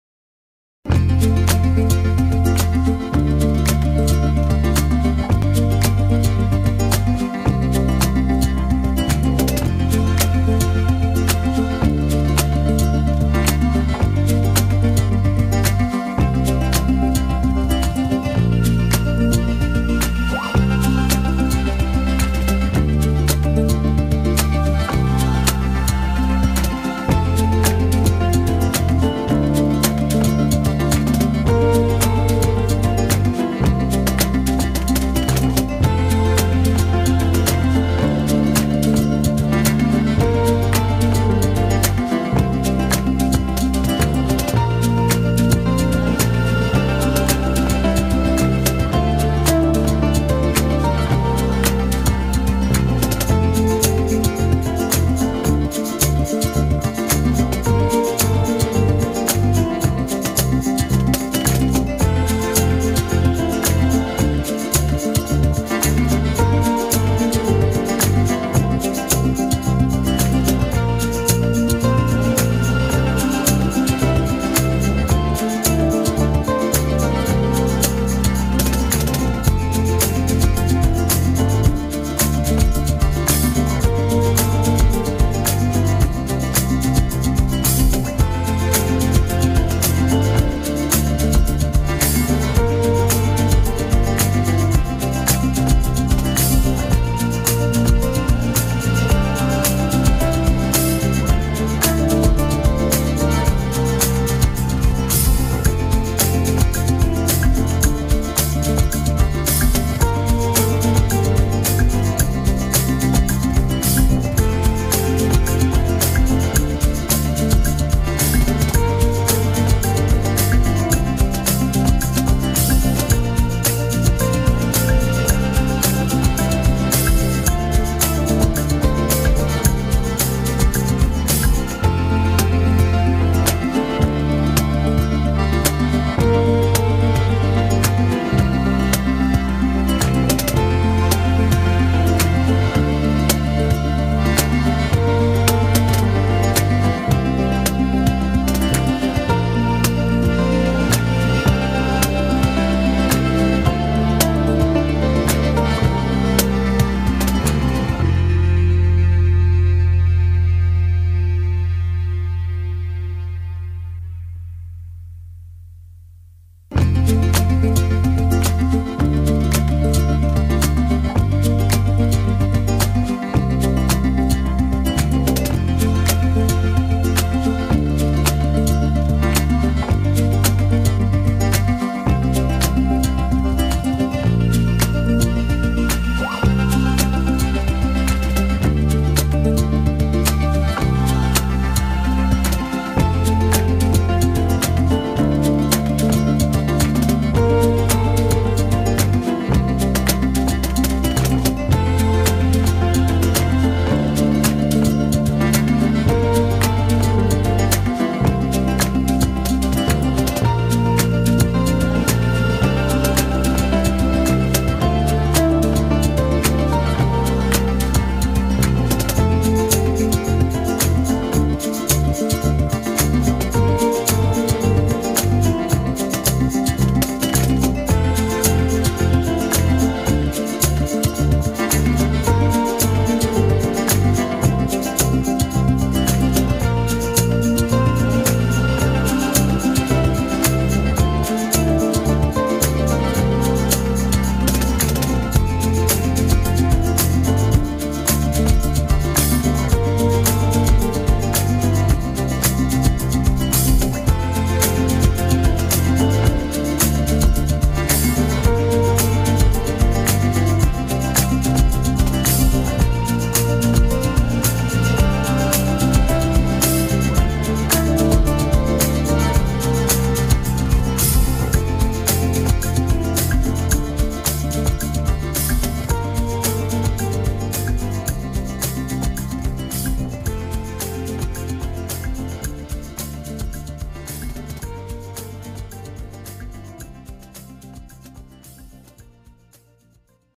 ALL NEW 2022 HONDA CB 500 X SPECIAL GREEN Pearl organic
The DOHC liquid-cooled parallel-twin engine uses a 67 mm x 66.8 mm square bore & stroke for the optimal combination of powerful low-end torque and excellent top-end power and performance.